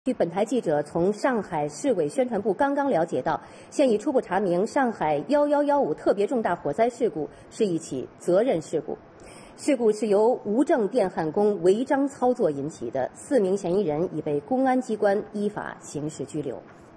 上海特大火灾事故原因初步查明 （替罪羊篇）
上海胶州路高楼火灾致53人遇难 国务院展开调查